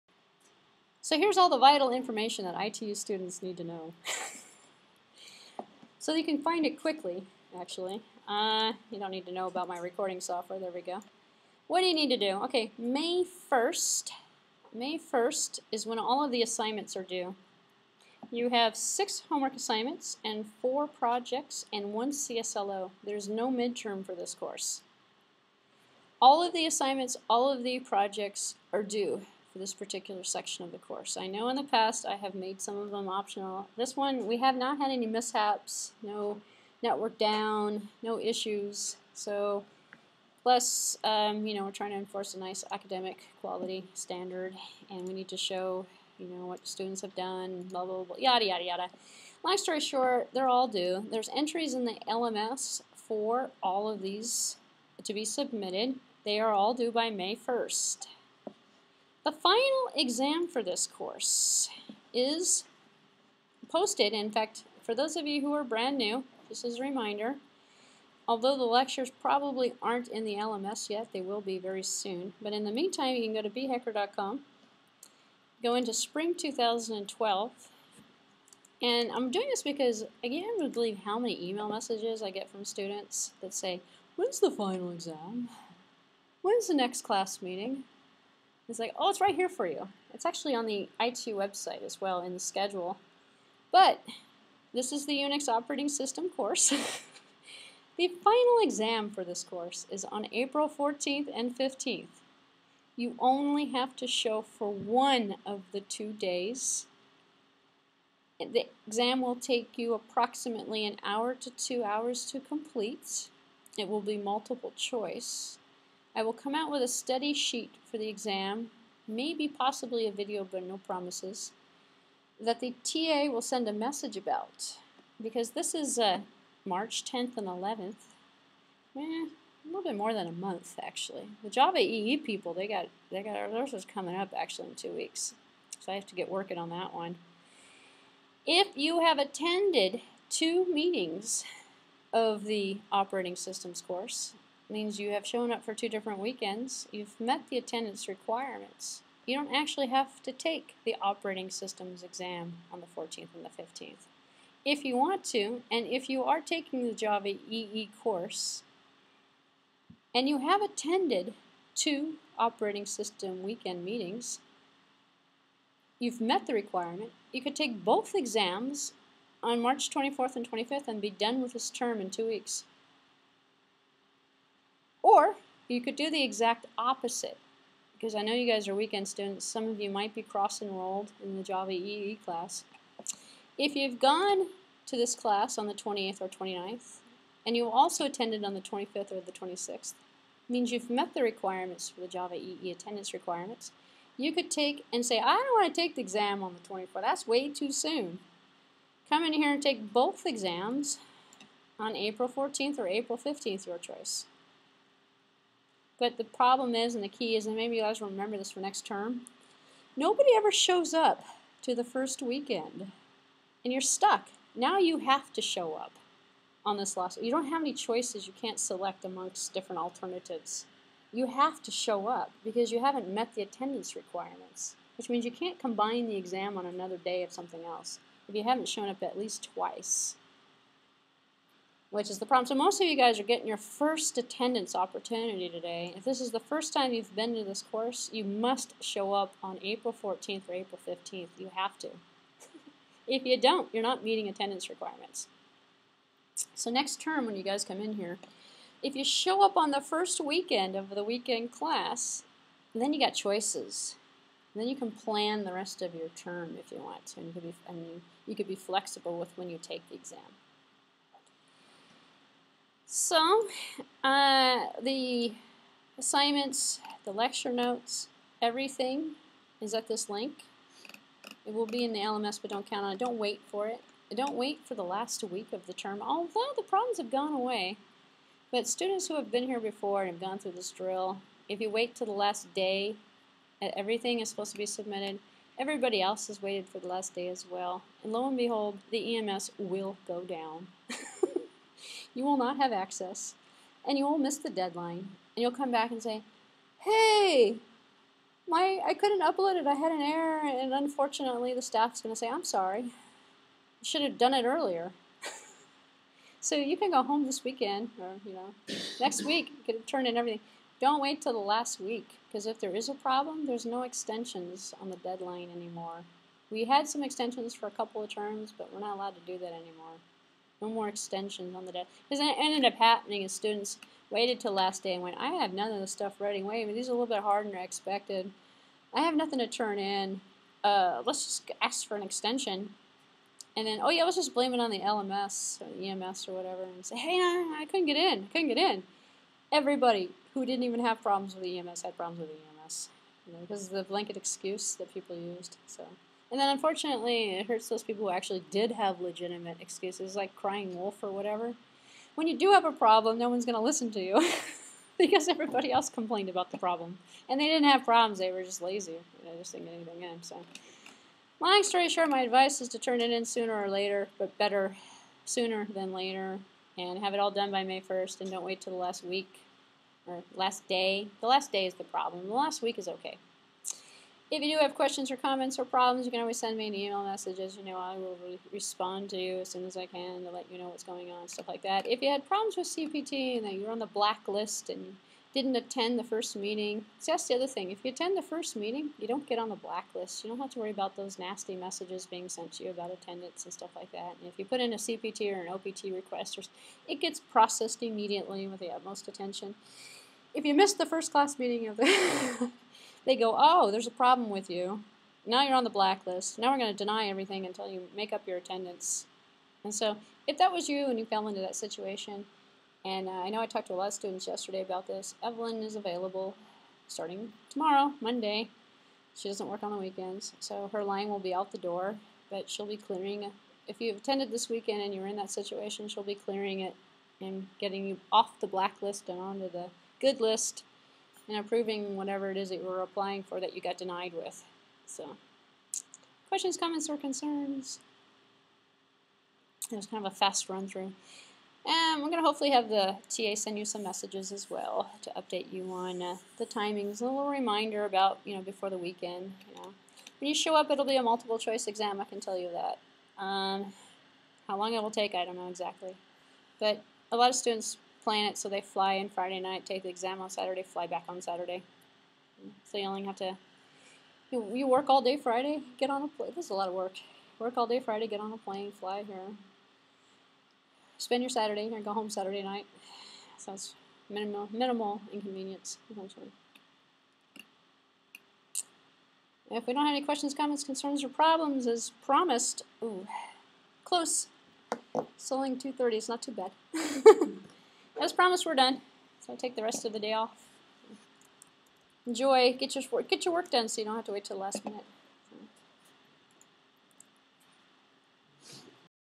Unix OS - Lecture 13
This lecture is intended for students taking the ITU Unix OS weekend class. General YouTube viewers are not going to find this useful. The video contains a summary of some important dates and information related to the assignment submissions and class requirements. It contains the closing remarks and information related to the next class meeting and the final exam.